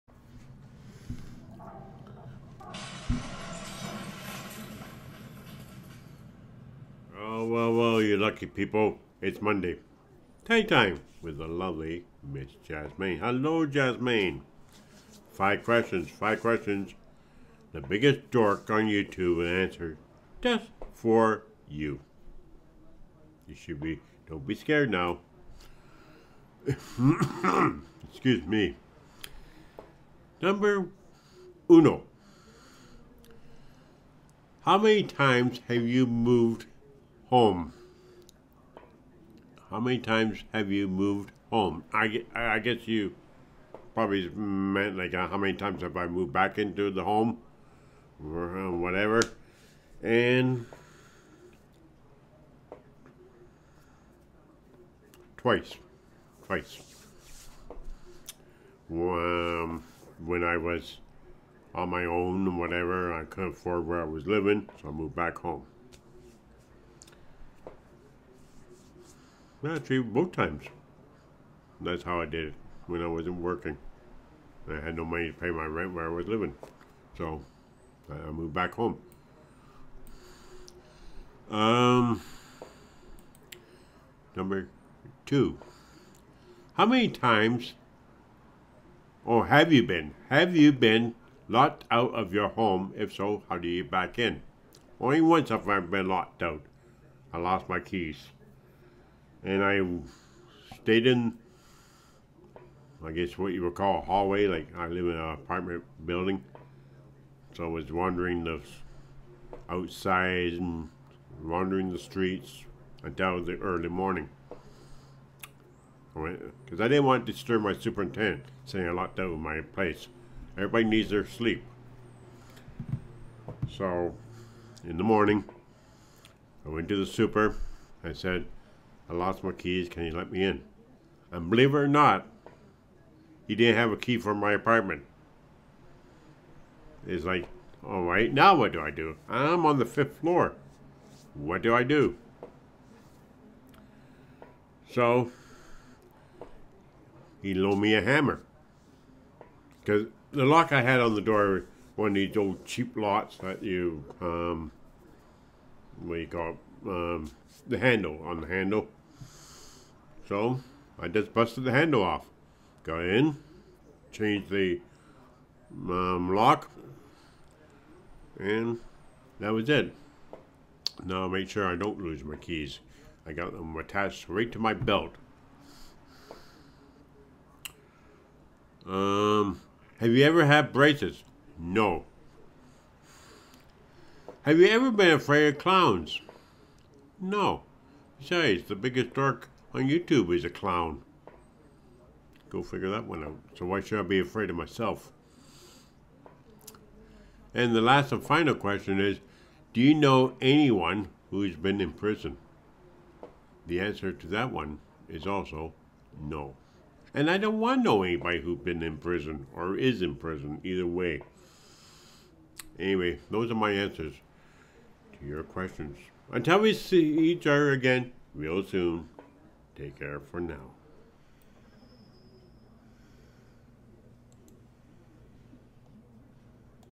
RE: Monday Weekly Tag (August 10 2020)
QUESTIONS:
1 … How many times have you moved home
2 … Have you been locked out of your home …  If so how did you get back in
3 … Have you ever had braces
4 … Ever been afraid of clowns
5 … Do you know anyone who been in prison